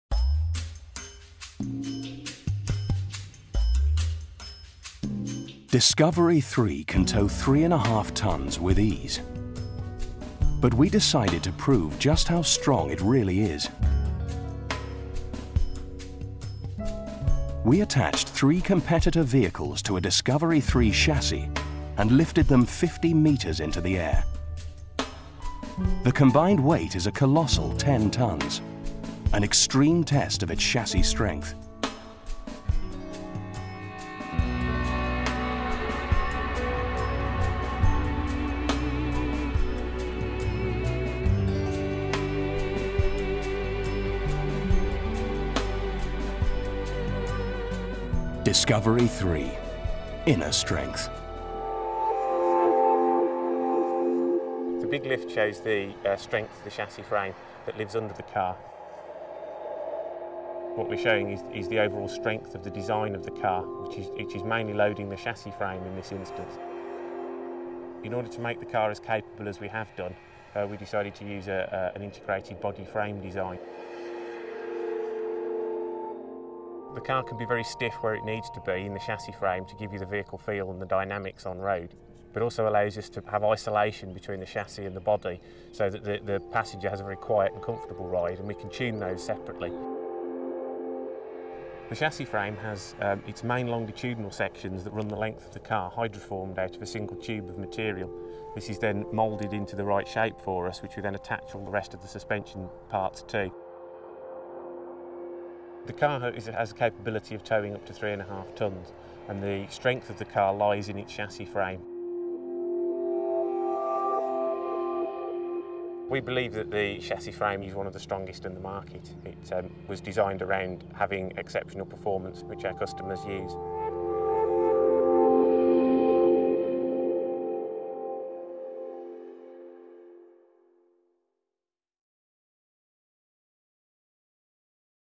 Land Rover Discovery 3 strong video [HQ]
Discovery 3 is powered by an advanced 2.7 litre TDV6 common-rail diesel
Discovery 3 features four-corner independent Air suspension with electronic cross-linking and variable ride height
Discovery 3 features seven full-size adult seats in three rows
CO2 emissions offset for the first 45,000 miles of use via the world class provider Climate Care
Land Rover's Terrain Response® system provides expert settings for all driving conditions